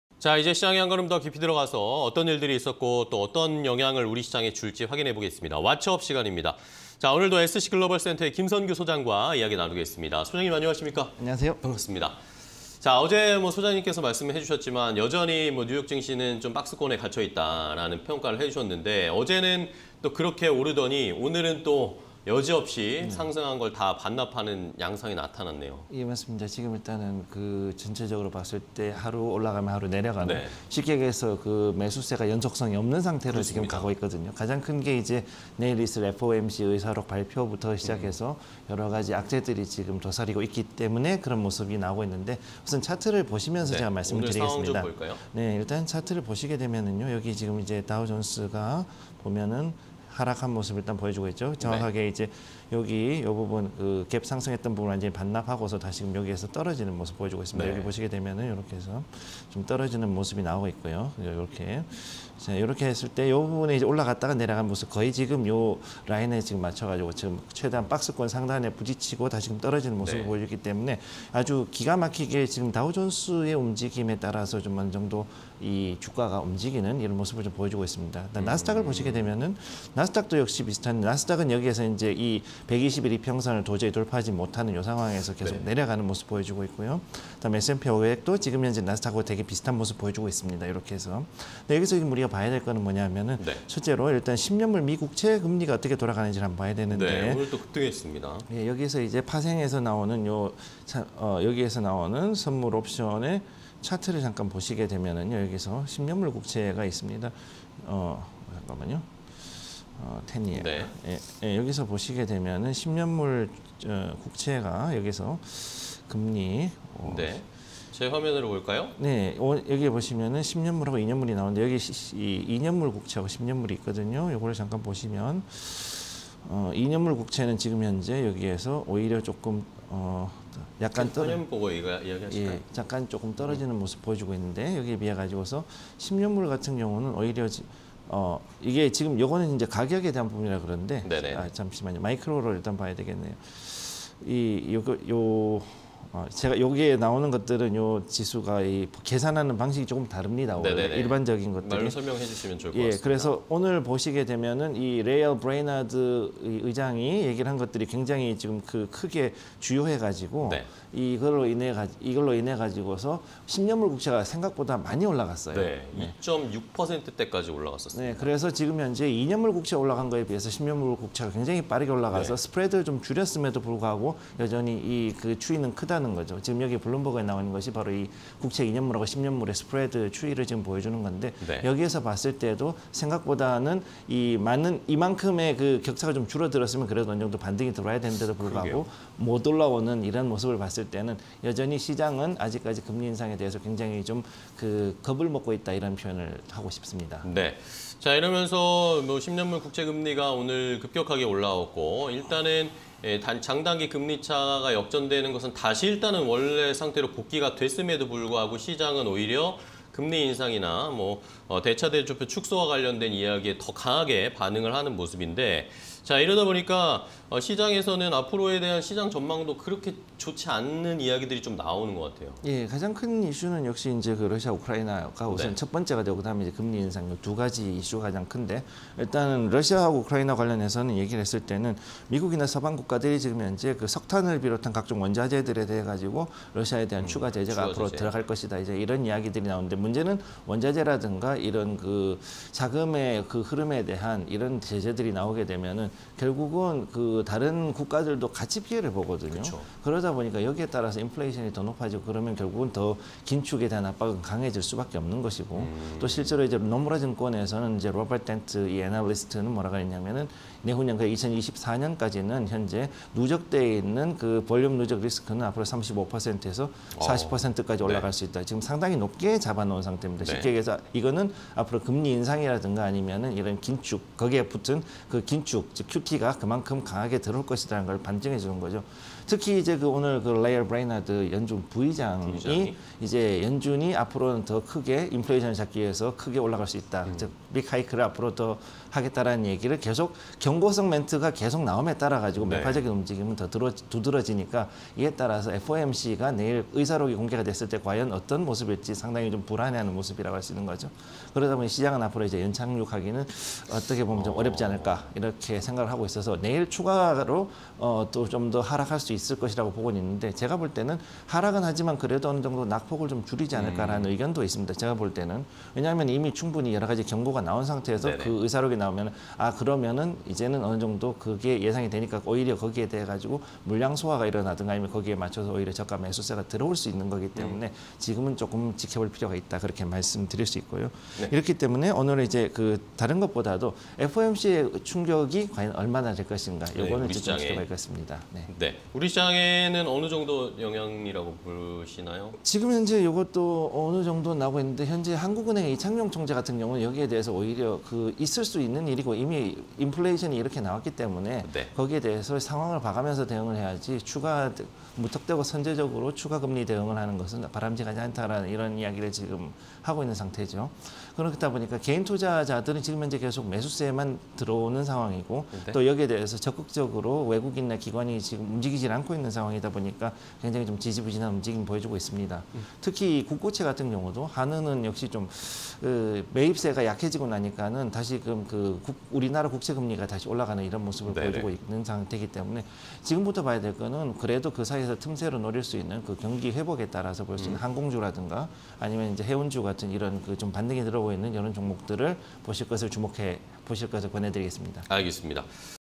달러 패권 위해 희생되는 증시… 강한 금리 인상은 올 것인가?_김선규의 왓츠업 (20220406)
# 뉴욕 3대 지수, 하락 마감
- 레이얼 브레이너드 연준 부의장, 매파적 발언
- 3대 지수, 연속성 없는 수급으로 차익실현 매물 매도세↑
- 달러인덱스, 100달러 대 향해 강한 상승세 보여

# 美 국채 10년 물, 3일 연속 상승… 여전한 역전 상황

# 수요일 'FOMC 의사록 공개' 주의보
- 노무라 증권의 로버트 덴트, "2024년까지 불황 누적 리스크 35-40% 달할 것"
- 미국·서방 국가들, 러시아 원자재 관련 추가 제재 준비: 러시아 유로채 오히려, 가격 상승
- 연준의 매파적 움직임 두드려: 시장, 연착륙 달성 어려울 것으로 예상
- FOMC 의사록 공개 후 금리 인상 관련 충격 주의

Q. 국내 증시 영향은?
- 개인투자자 매수세
- 한은 국고채 매입, 큰 영향 주지 못한 채 다시 금리 상승시켜
- 경기회복에 따라 반등 들어오고 있는 항공·해운 관련주 주목